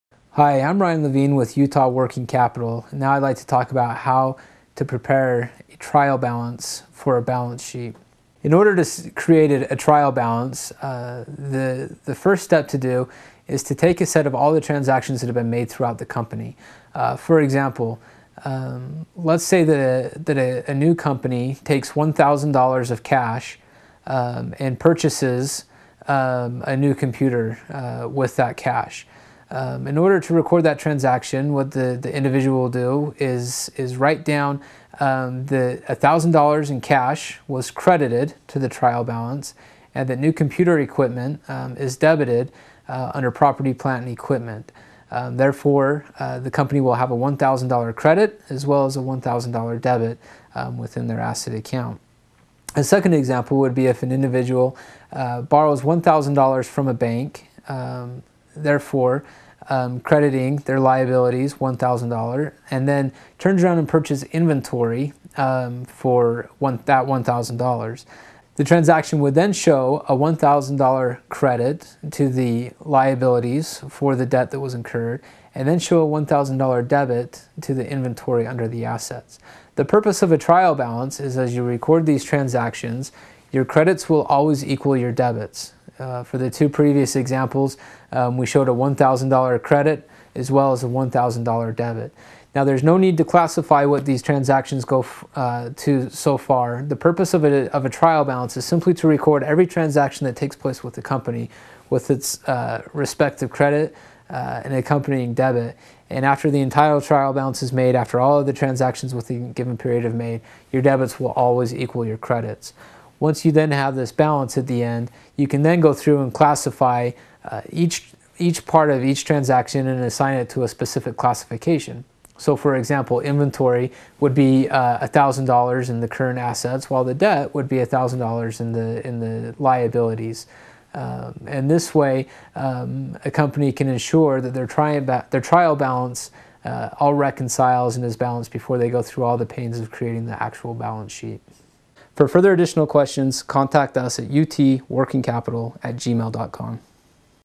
Balance Sheets : How to Prepare a Trial Balance Sheet
Creating a trial balance for a balance sheet involves taking a set of all transactions that a company has made, recording them and making sure that the debits are equal to the credits. Assign each part of a transaction to a specific classification with help from an experienced accountant in this free video on balance sheets.

Expert: Ryan Lavigne
Bio: Ryan Lavigne has over seven years of experience working in accounting and finance.